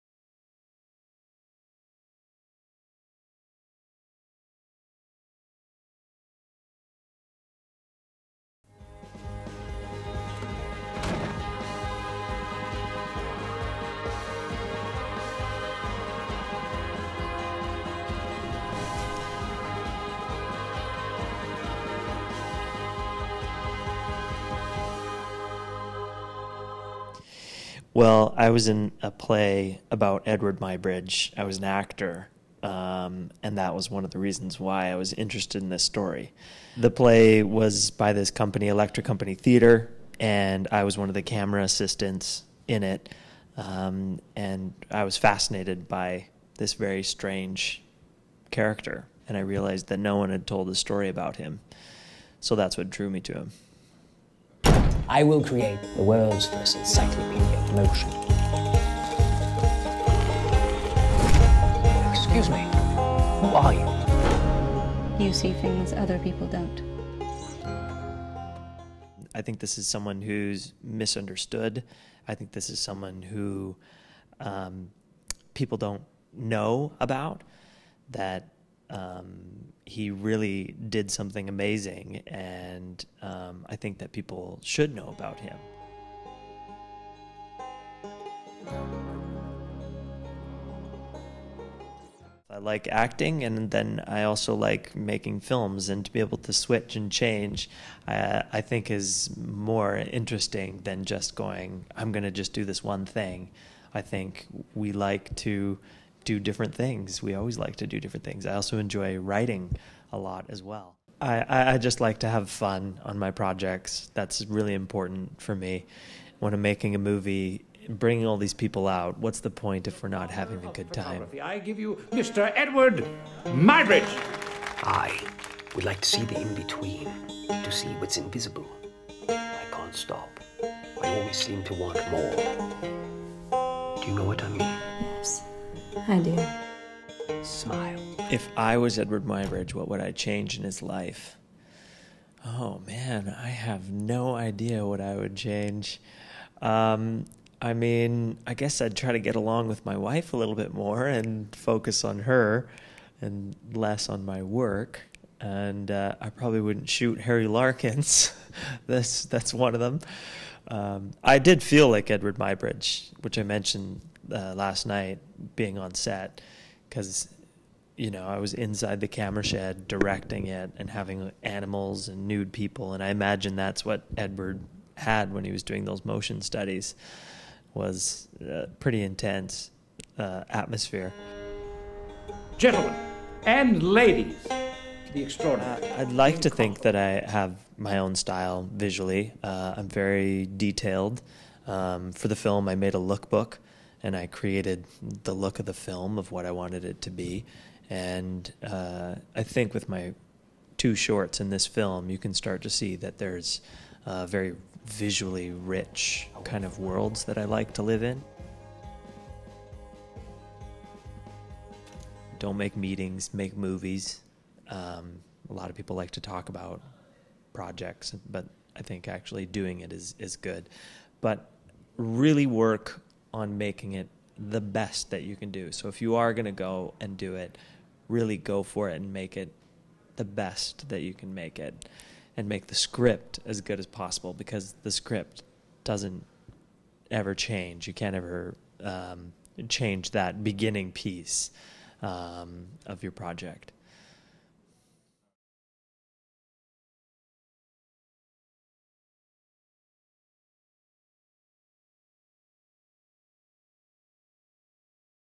Eadweard
Kyle Rideout maakte een biopic over Edward Muybridge en deelt hem enthousiast met het festivalpubliek in Gent. Marnick Buys deelt zijn enthousiasme.